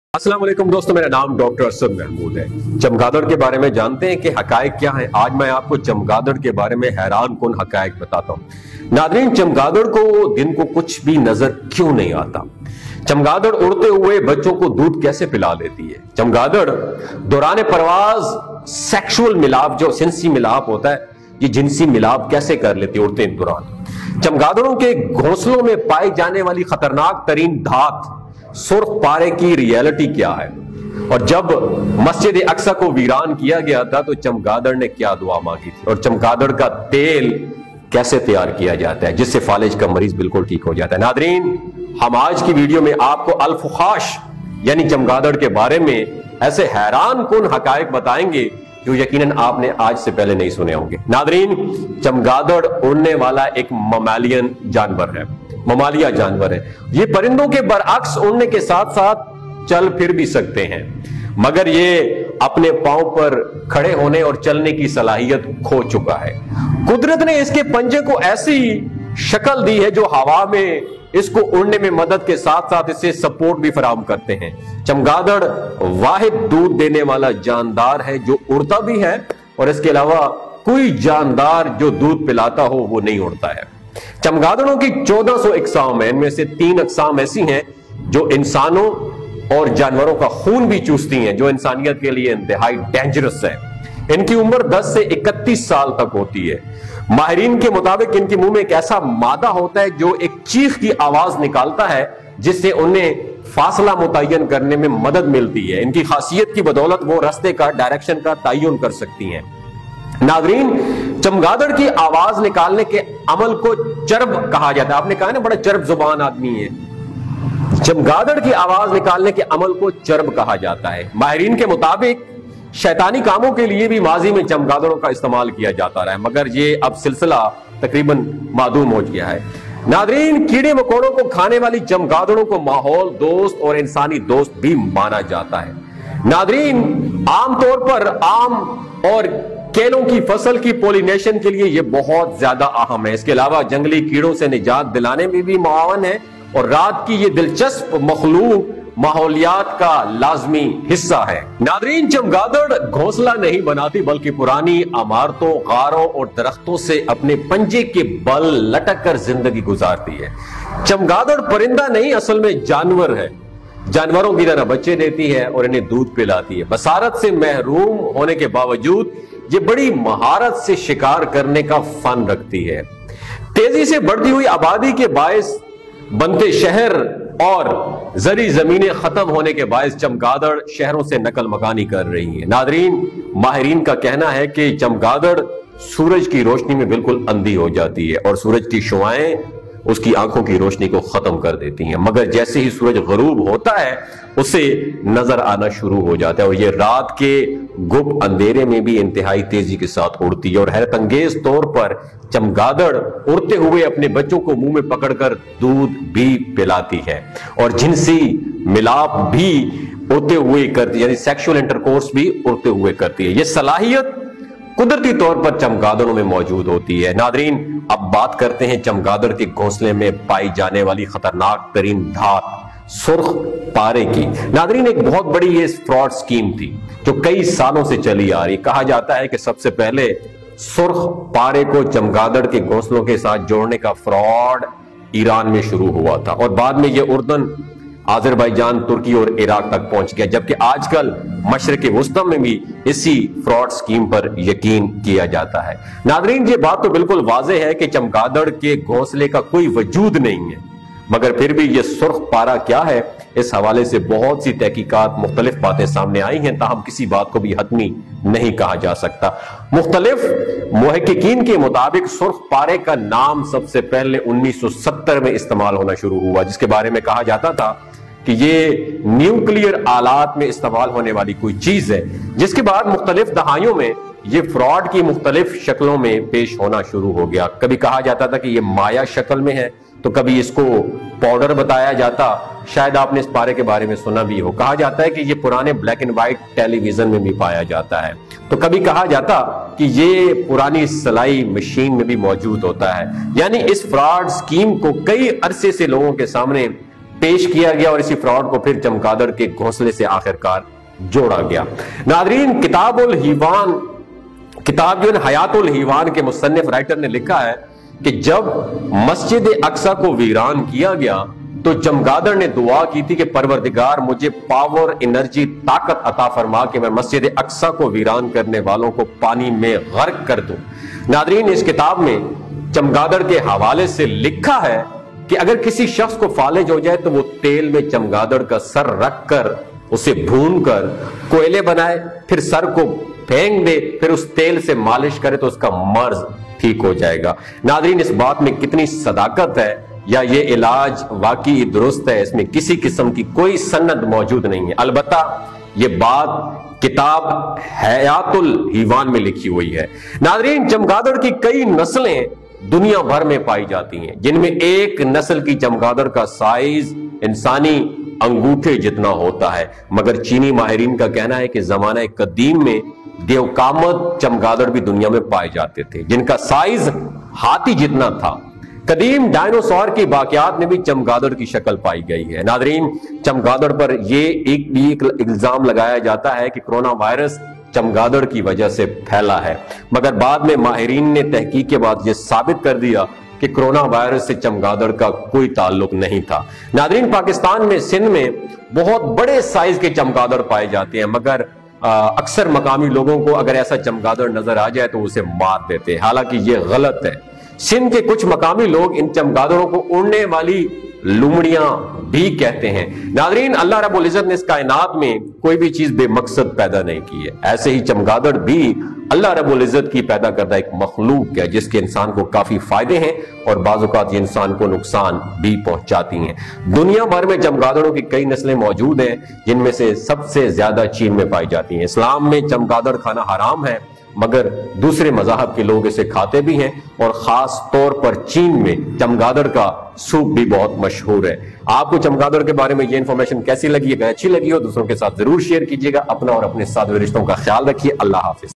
How was the bat born ? चमगादड़ का जन्म कैसे हुआ? #| By Dr Asad Mehmood Urdu| Hindi
چمگادڑ کی پیدائش کیسے ہوئی ؟ دن کو کیوں اندھی ہوتی ہے ؟
جب مسجد اقصی کو ویران کیا گیا تو چمگادڑ نے کیا دعا مانگی ؟